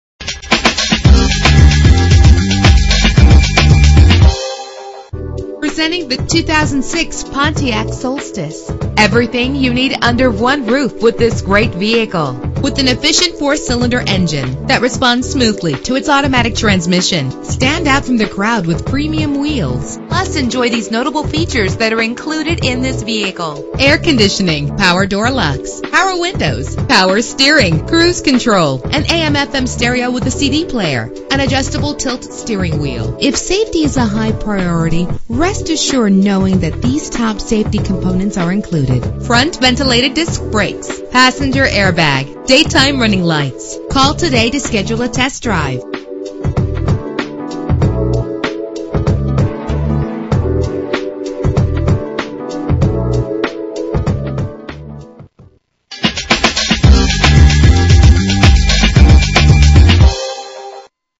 2006 Pontiac Solstice - PEARLAND TX
Year: 2006
Make: Pontiac
Model: Solstice
Trim: Base
Engine: 2.4 liter inline 4 cylinder 16 valve
Transmission: Automatic with Overdrive
Color: Gray
Mileage: 62391
Address:
5719 BROADWAY